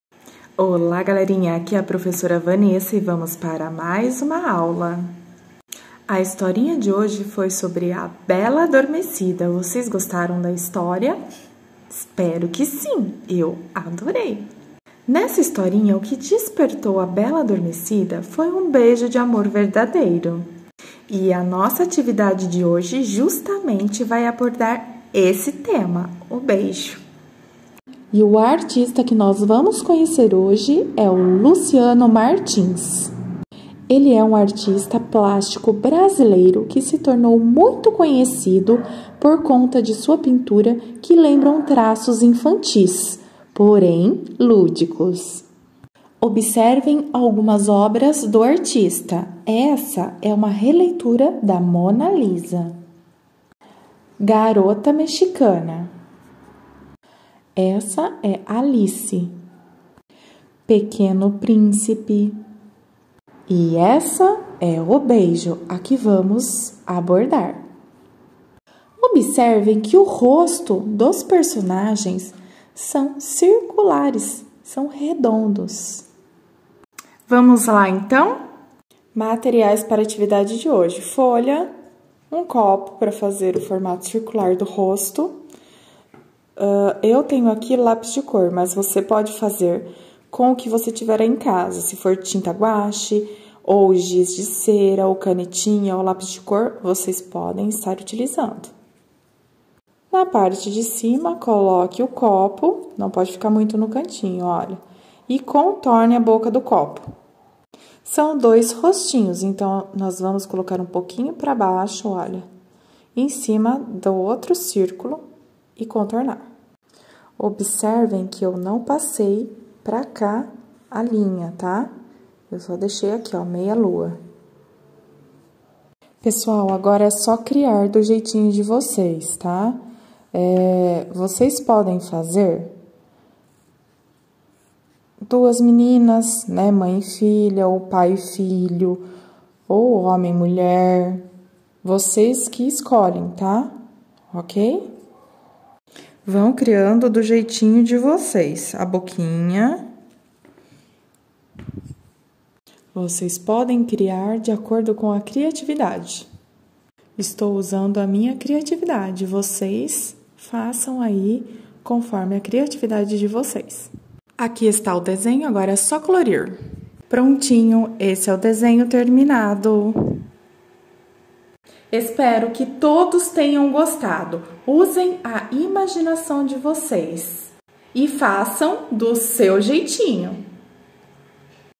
ATIVIDADE- O BEIJO - PROFESSORA VANESSA MESSIAS
Quando a criança desenha, cria pontes entre o mundo real e o imaginário, expressando suas concepções e percepções do mundo no qual está inserida. Além disso, o desenho permite à criança retratar em diferentes dimensões, suas experiências pessoais em busca da sua própria identidade.
Inspirado na obra de Luciano Martins, artista plástico brasileiro que se tornou muito conhecido por conta de sua pintura que lembram traços infantis, porém lúdicos.